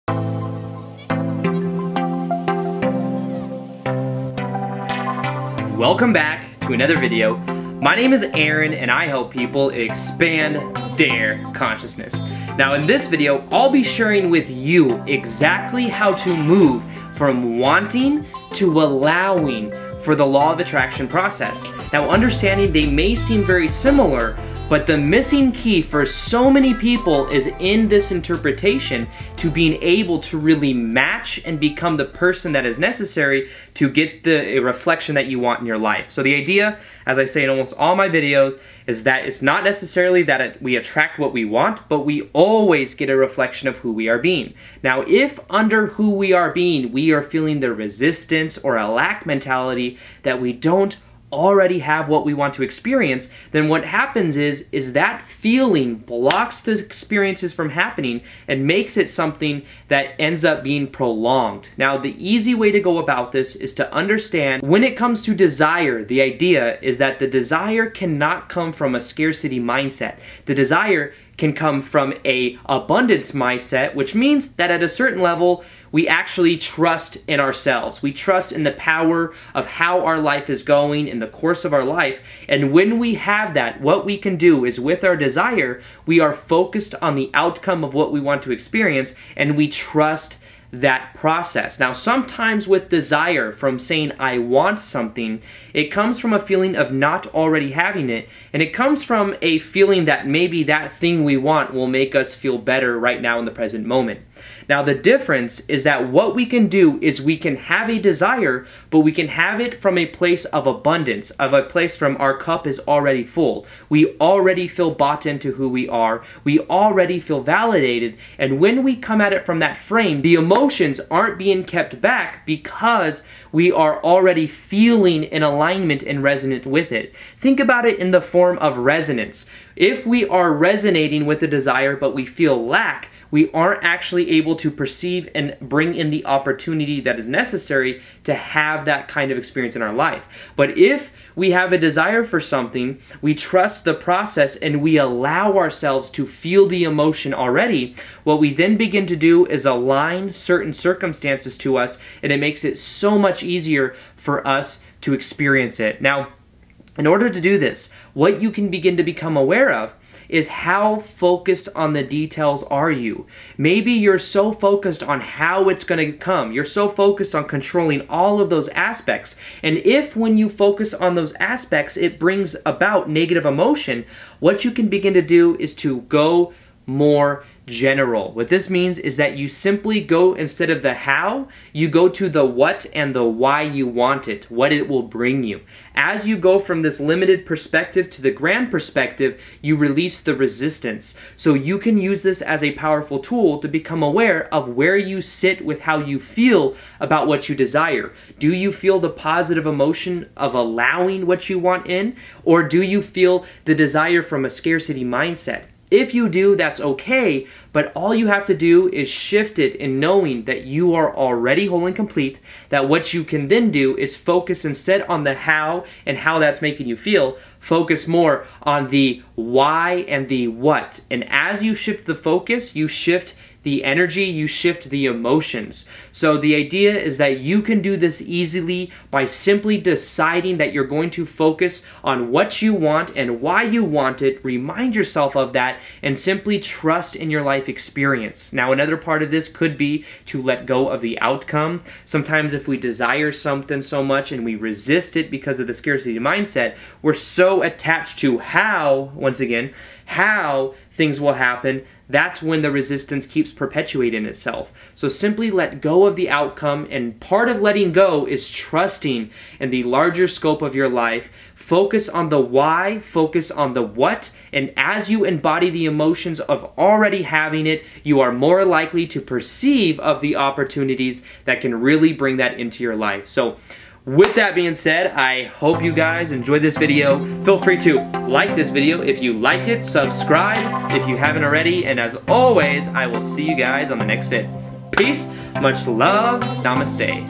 Go From Wanting to Allowing By Doing This (Missing Key)
This video will show you Exactly how to go from wanting to allowing.  I will show you an easy process that will release resistance so you can begin to align with the outcomes you want to accomplish.  Transcript below...
"Welcome back to another video. My name is Aaron, and I help people expand their consciousness. Now in this video I'll be sharing with you exactly how to move from wanting to allowing for the law of attraction process. Now understanding they may seem very similar, but the missing key for so many people is in this interpretation to being able to really match and become the person that is necessary to get a reflection that you want in your life.
"The idea, as I say in almost all my videos, is that it's not necessarily that we attract what we want, but we always get a reflection of who we are being.
"Now if under who we are being we are feeling the resistance or a lack mentality that we don't already have what we want to experience, then what happens is that feeling blocks the experiences from happening and makes it something that ends up being prolonged.
"Now the easy way to go about this is to understand, when it comes to desire, the idea is that the desire cannot from a scarcity mindset. The desire can come from a abundance mindset, which means that at a certain level we actually trust in ourselves. We trust in the power of how our life is going in the course of our life. When we have that, what we can do is, with our desire, we are focused on the outcome of what we want to experience and we trust that process.
"Now sometimes with desire, from saying I want something, it comes from a feeling of not already having it. It comes from a feeling that maybe that thing we want will make us feel better right now in the present moment.
"Now the difference is that what we can do is we can have a desire but we can have it from a place of abundance, of a place from our cup is already full. We already feel bought into who we are. We already feel validated. When we come at it from that frame, the emotions aren't being kept back because we are already feeling in alignment and resonant with it.
"Think about it in the form of resonance. If we are resonating with the desire but we feel lack, we aren't actually able to perceive and bring in the opportunity that is necessary to have that kind of experience in our life. But if we have a desire for something, we trust the process, and we allow ourselves to feel the emotion already, what we then begin to do is align certain circumstances to us. It makes it so much easier for us to experience it.
"Now in order to do this, what you can begin to become aware of is how focused on the details are you. Maybe you're so focused on how it's going to come. You're so focused on controlling all of those aspects. If, when you focus on those aspects, it brings about negative emotion, what you can begin to do is to go more general. What this means is that you simply go instead of the how, you go to the what and the why you want it, what it will bring you.
"As you go from this limited perspective to the grand perspective, you release the resistance. You can use this as a powerful tool to become aware of where you sit with how you feel about what you desire. Do you feel the positive emotion of allowing what you want in or do you feel the desire from a scarcity mindset? If you do, that's okay, but all you have to do is shift it in knowing that you are already whole and complete, that what you can then do is focus instead on the how and how that's making you feel, focus more on the why and the what. As you shift the focus, you shift the energy and you shift the emotions.
"The idea is that you can do this easily by simply deciding that you're going to focus on what you want and why you want it. Remind yourself of that and simply trust in your life experience.
"Now another part of this could be to let go of the outcome. Sometimes if we desire something so much and we resist it because of the scarcity mindset, we're so attached to how, once again, how things will happen. That's when the resistance keeps perpetuating itself. Simply let go of the outcome. Part of letting go is trusting in the larger scope of your life. Focus on the why. Focus on the what. As you embody the emotions of already having it, you are more likely to perceive of the opportunities that can really bring that into your life. With that being said, I hope you guys enjoyed this video. Feel free to like this video. Peace, much love, namaste.

This video is about how to Go From Wanting to Allowing By Doing This (Missing Key)